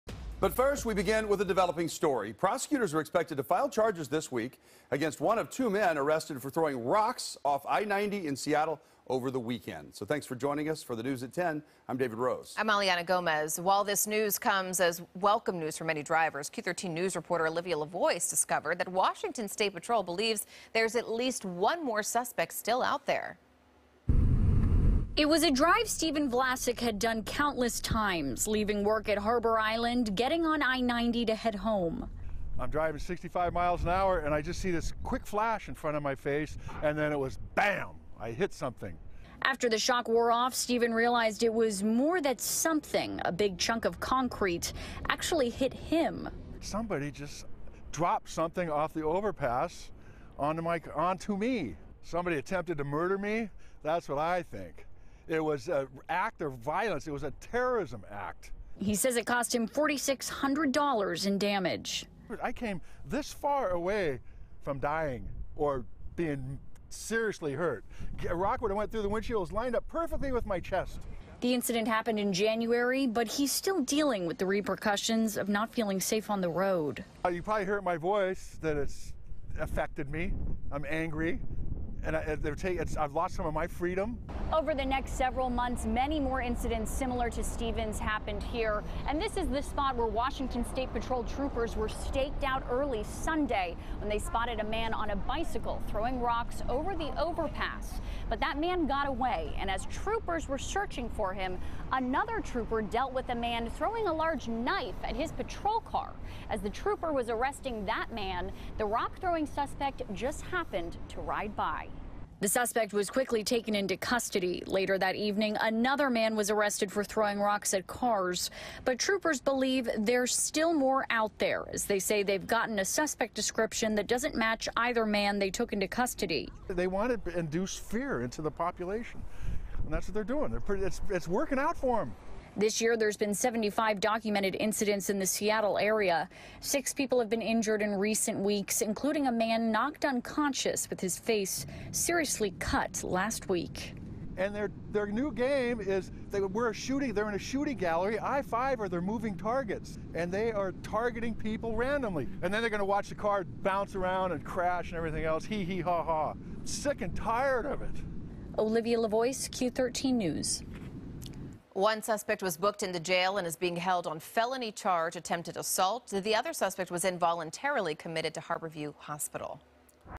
Two men arrested for throwing rocks at cars on freeway
Washington State Patrol has arrested a man who allegedly threw rocks at cars from an overpass near I-90 in Seattle.